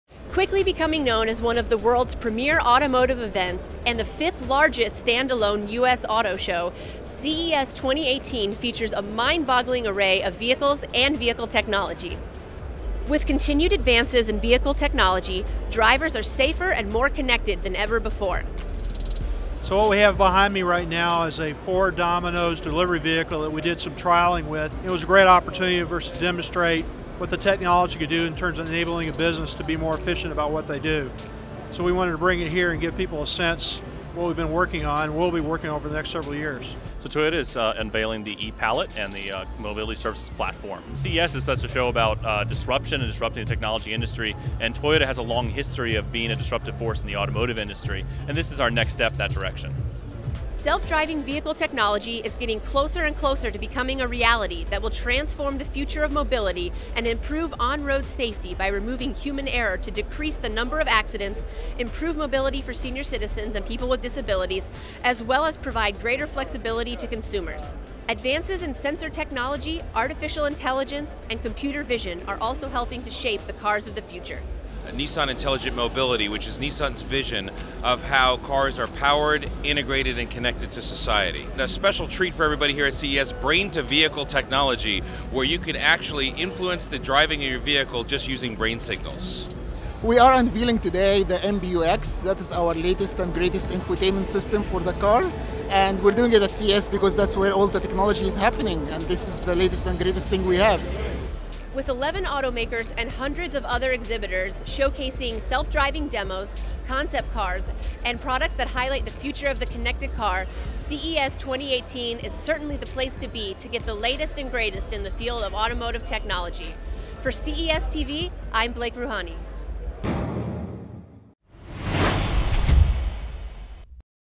CESTV 2018: Check Out Vehicle Technology on the Show Floor
CES 2018 is one of the world's premiere automotive events. Check out the show floor and hear from some exhibitors innovating vehicle technology.